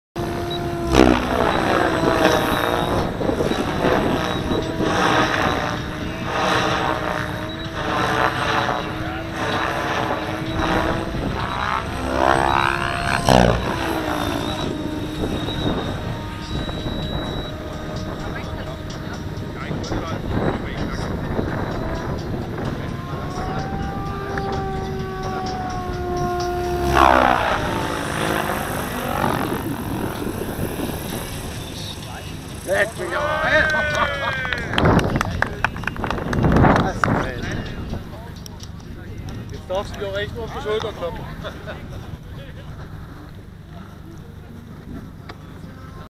Compass 7HV inverted Autorotation, Jet Landing, Uli Westenfelder
Compass 7HV beim Team Taiga Treffen 2014